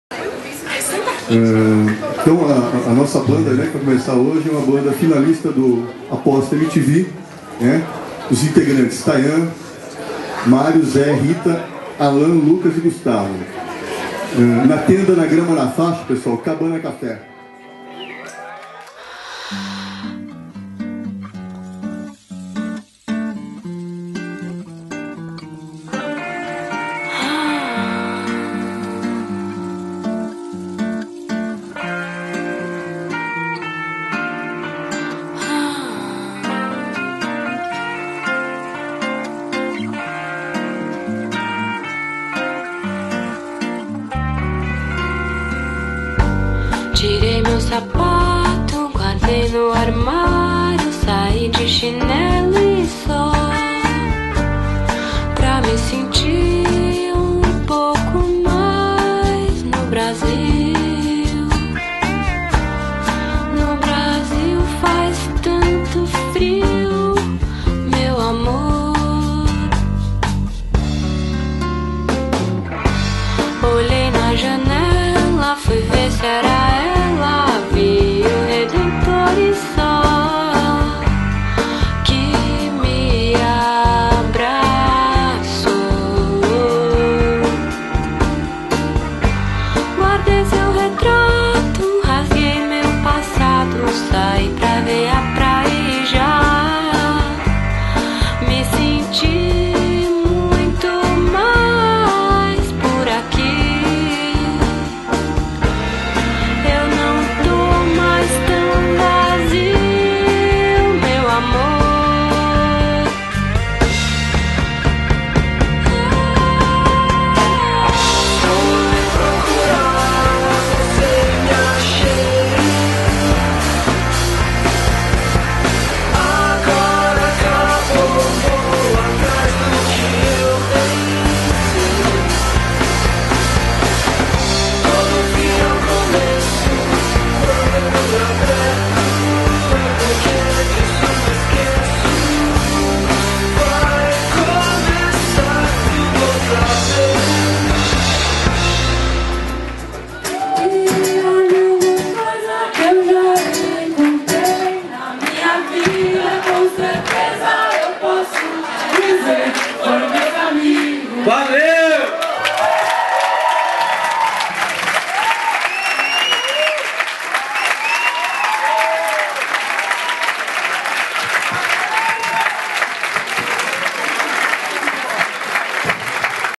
Cabana Café @ UFSCar Sorocaba
O projeto de extensão da Universidade Federal de São Carlos campus Sorocaba, Música na Cidade e o samscoletivo apresentam: Cabana Café.